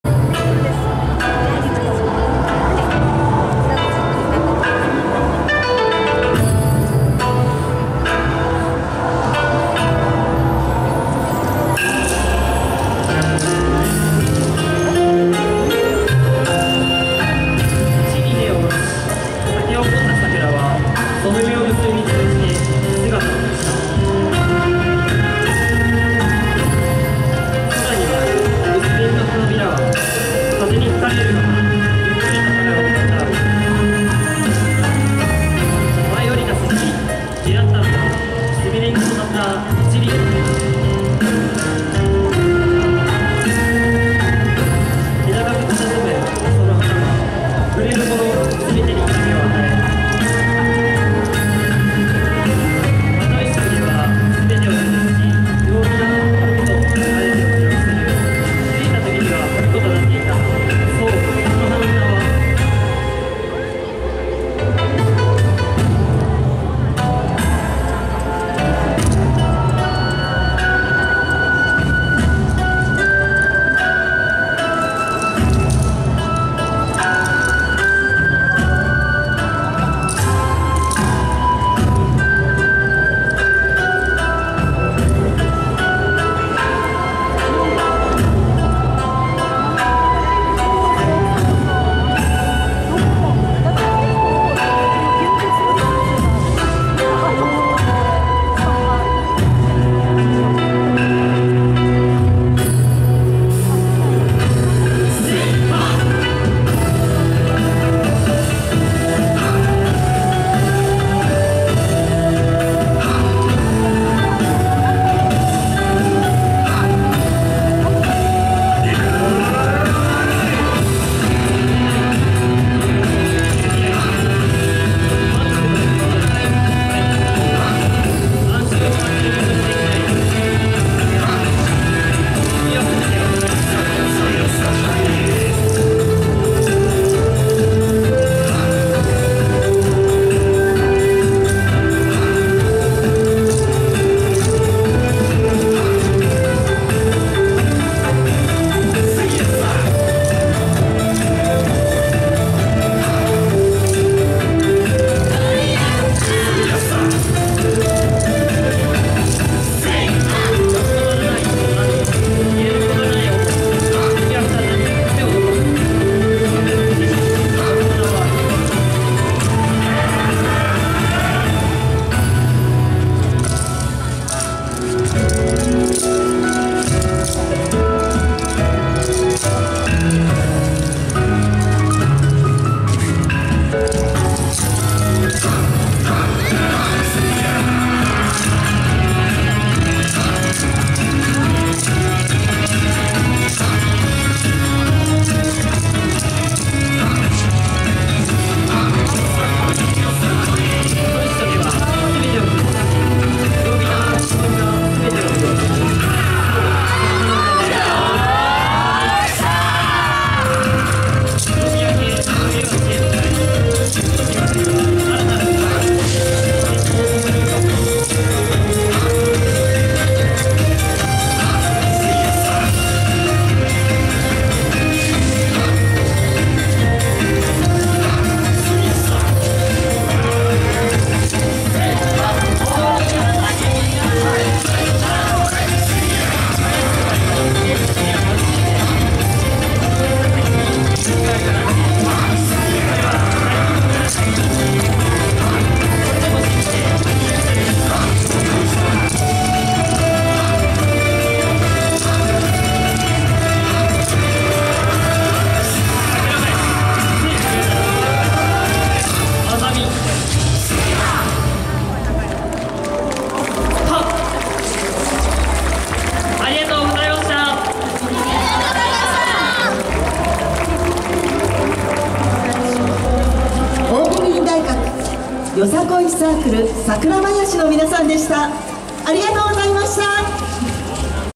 桜美林大学よさこいサークル桜囃子・東京よさこい前夜祭、2017年10月7日。
桜美林大学よさこいサークル桜囃子・東京よさこい前夜祭、アゼリア通り会場。狐